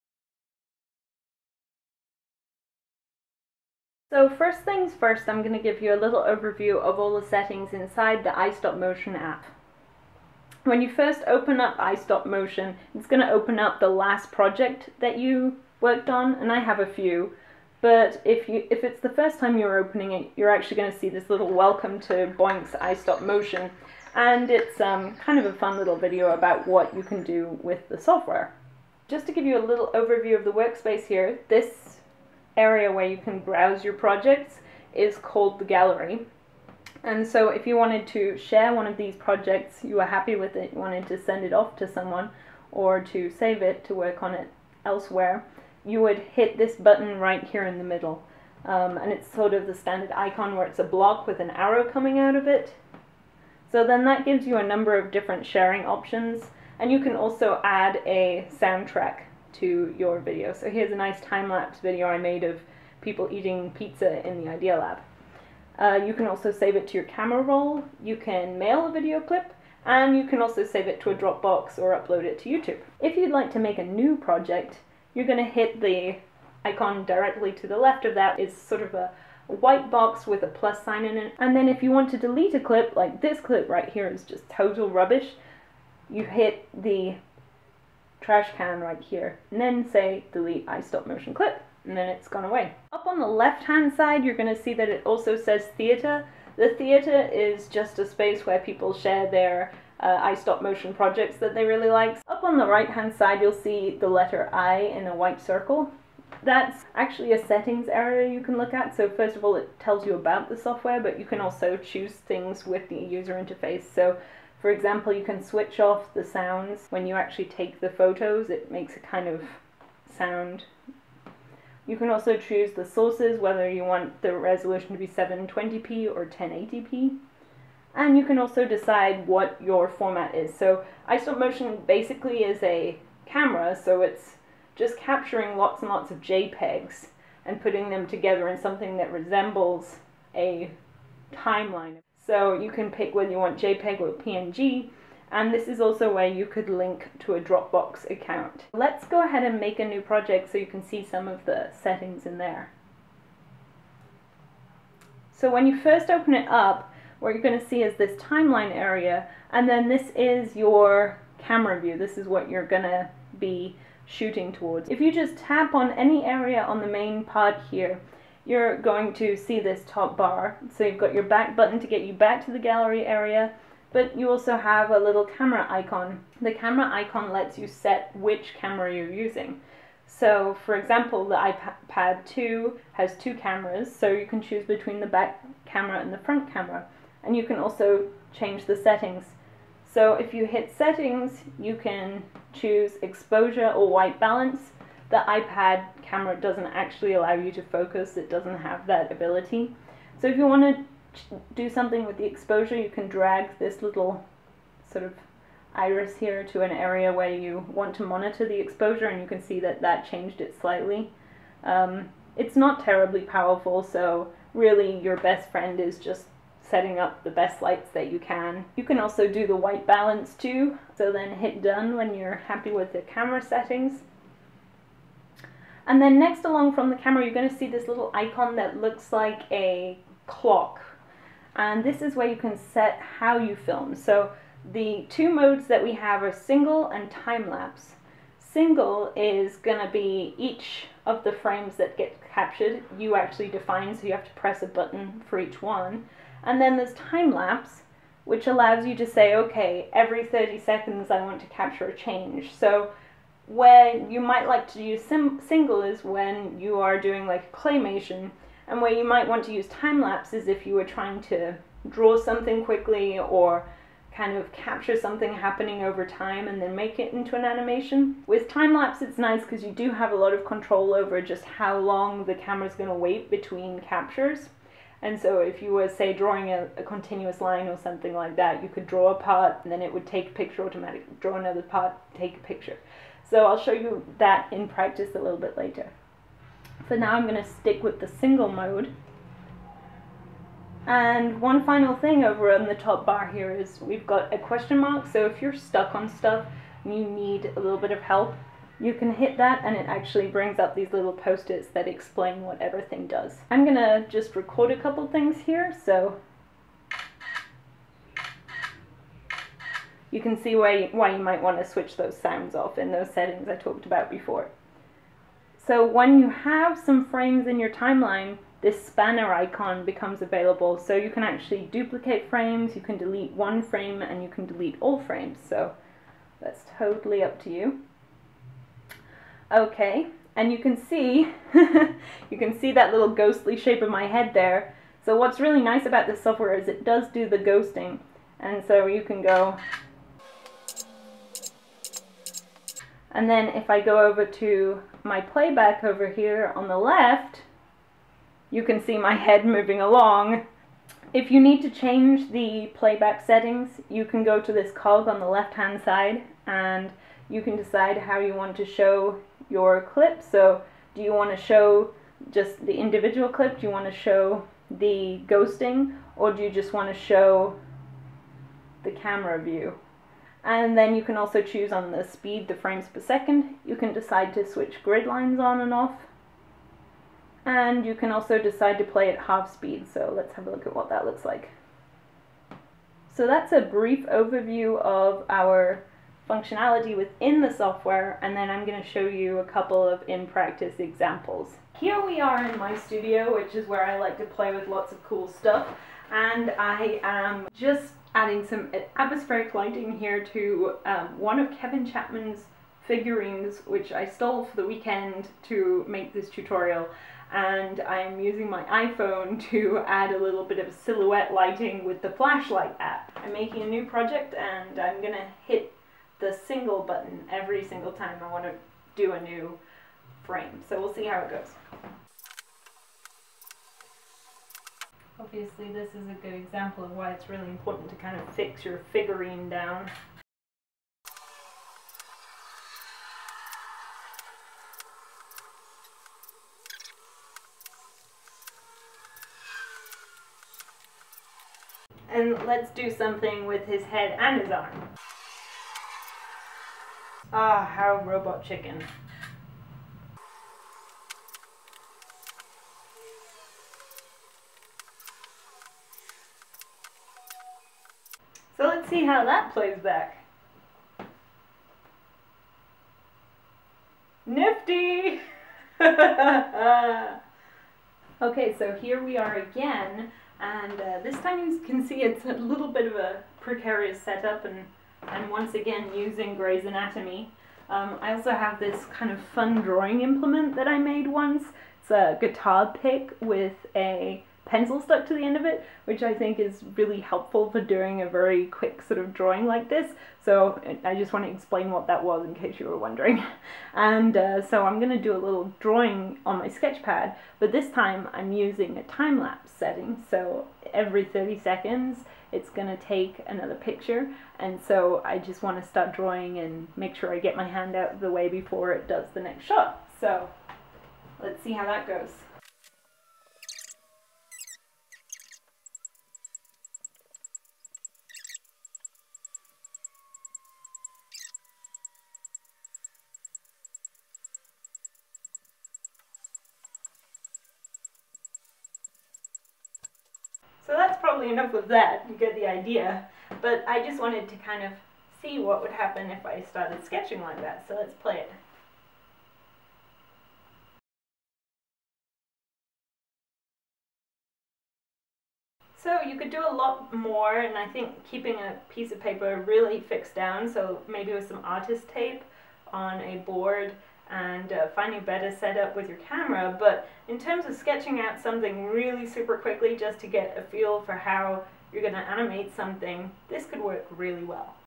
iStopmotion App Tutorial
This is a a tutorial for the iStopmotion animation app with some real-world examples.